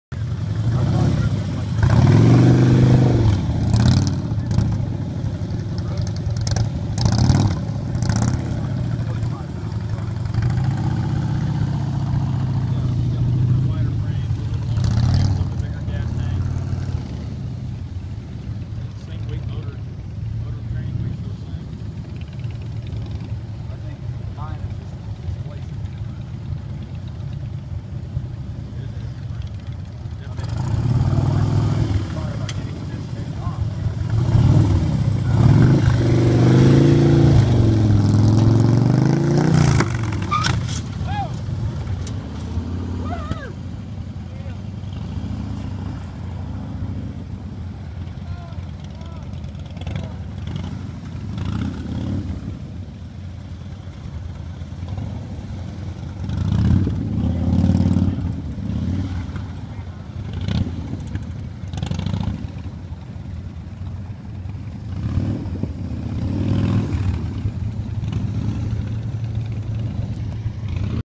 08/27/14 CRAZY ED'S PLP PLANO LAURA GORIN MAMED KEYHOLE (Drop, How to Pick Up Bike)
Good video showing how to step off a Motorcycle on a slow speed Parking Lot Practice (Motorcycle Skills Practice) and how to pick it up. Good job Laura..
Thanks to Joe and Laurie Talarico, Kevin Lovett Jr. and Richard Diamond for being right there for her.